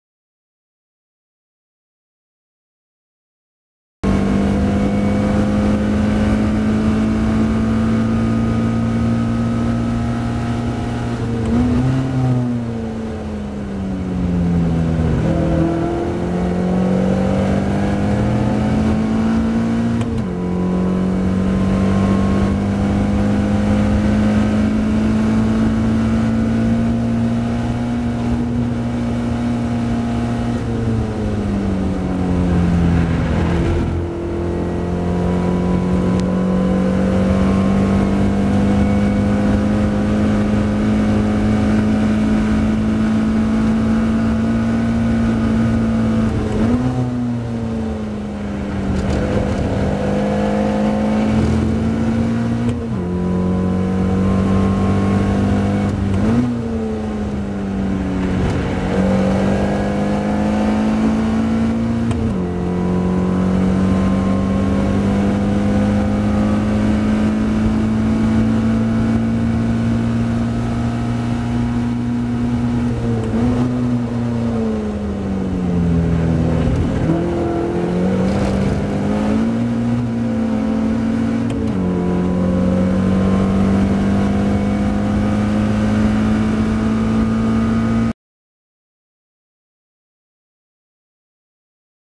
Ben Clucas Driving a Caterham Tracksport Car Around Donington National Circuit
Ben Clucas driving a Caterham Tracksport car around Donington National circuit, lapping in 1.20.1, 0.6 seconds under the lap record. If you are interested in driver coaching please